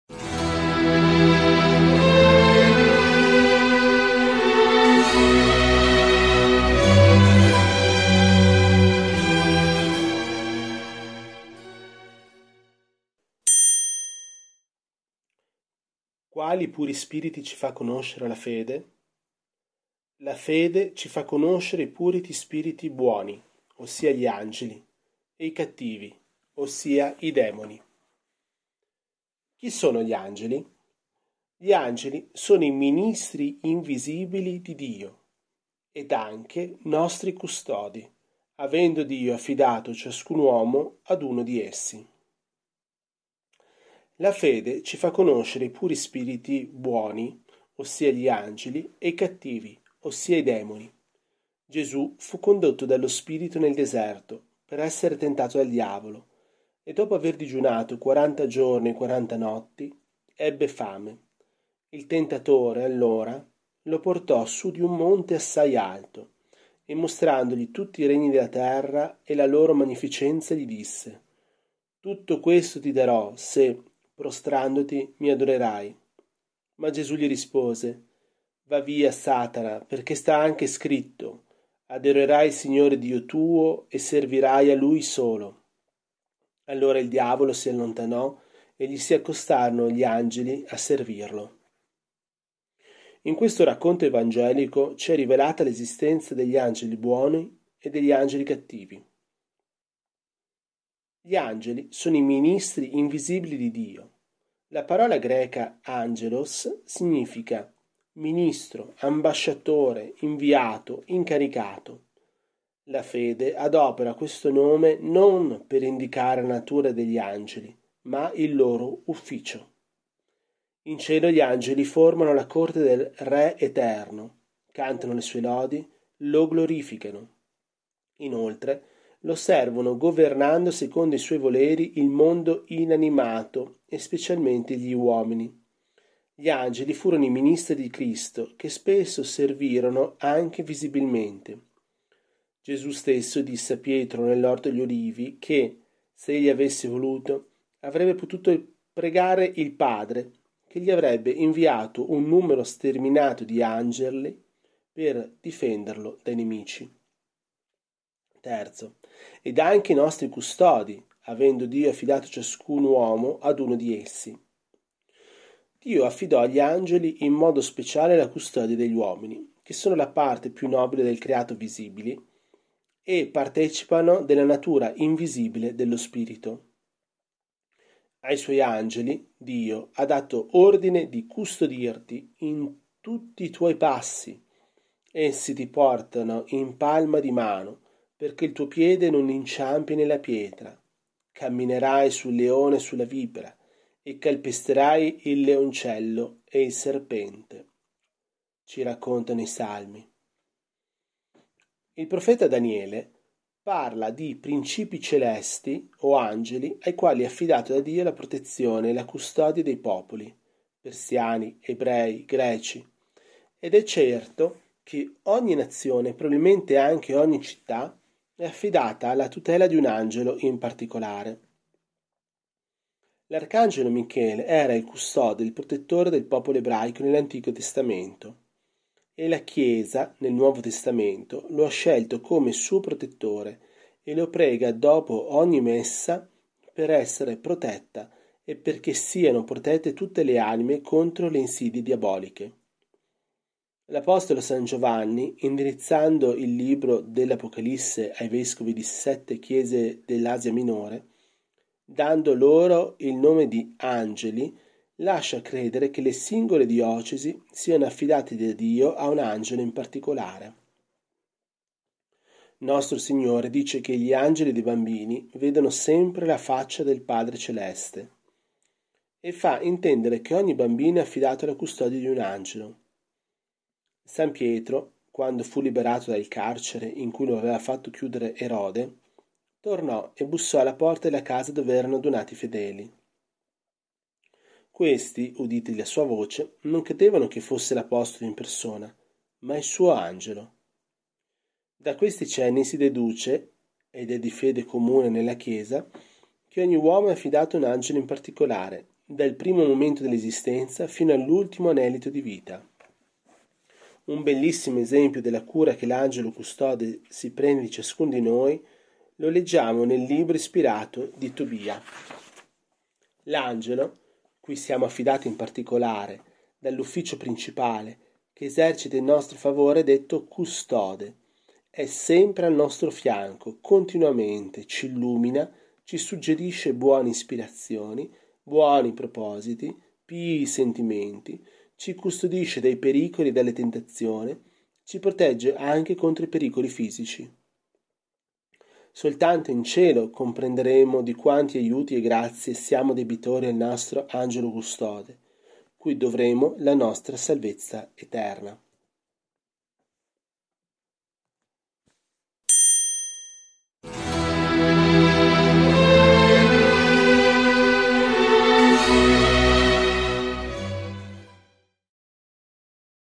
242 - Chi Sono gli Angeli
Questo testo è tratto dal libro Spiegazione del Catechismo di San Pio X di Padre Dragone.